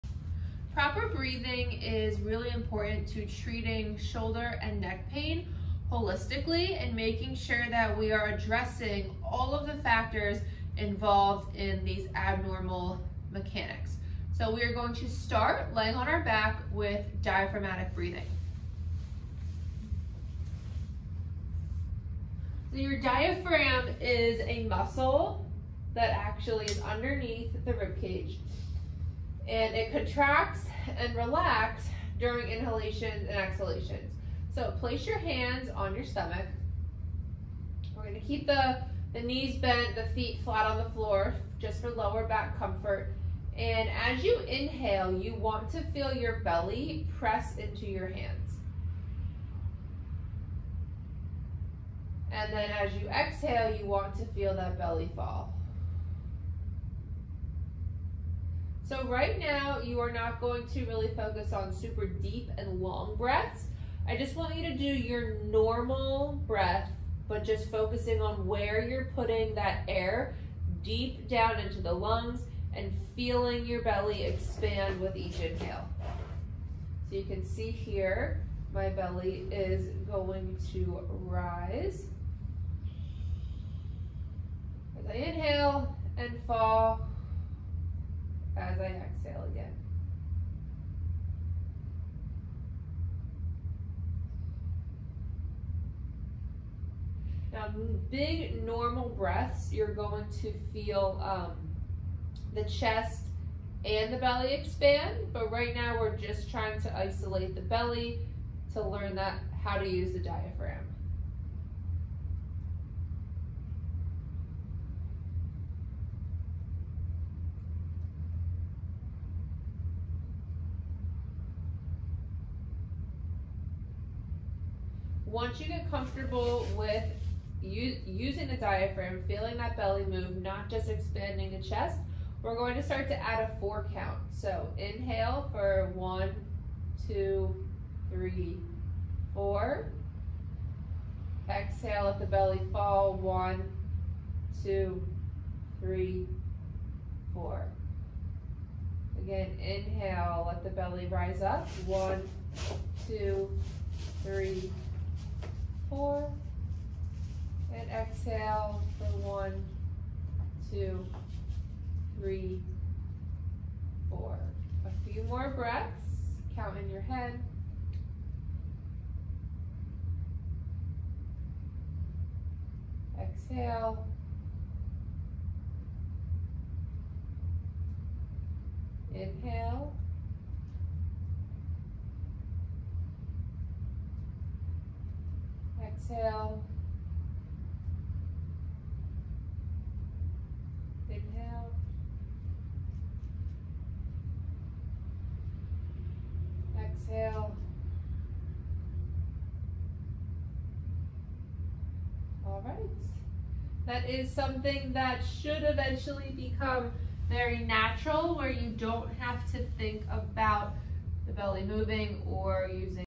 Diaphragmatic breathing
diaphragmatic breathing... literally the foundation of EVERYTHING.
neck pain
shoulder pain
back pain
hip pain
ANY PAIN

you need to be able to breath with your diaphragm. It stimulates the vagus nerve which can help calm the body and decrease pain. Proper function of the diaphragm is necessary for all the other muscles in your body to work the way they are meant to !

We offer a variety of guides specific to different body parts to help you start moving and feeling better TODAY!